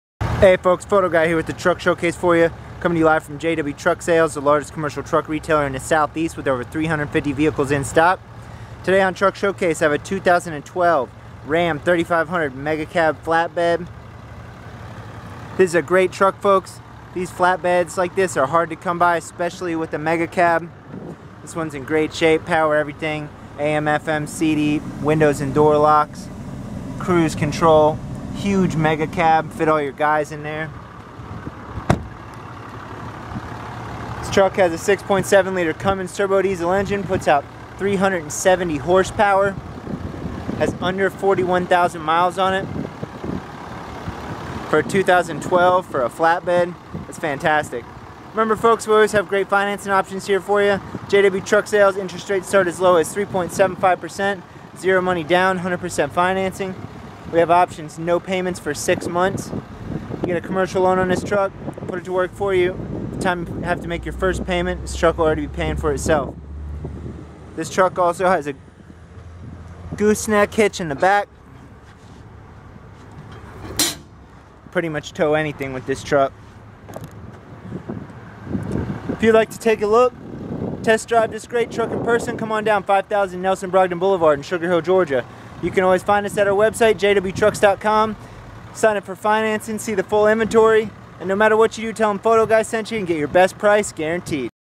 2012 RAM 3500 Crew Cab Flatbed - TRUCK SHOWCASE
This RAM is loaded and ready to work! If you need a great flatbed with low miles and a goose neck then you need to check this truck out. Take a look with Photo Guy!!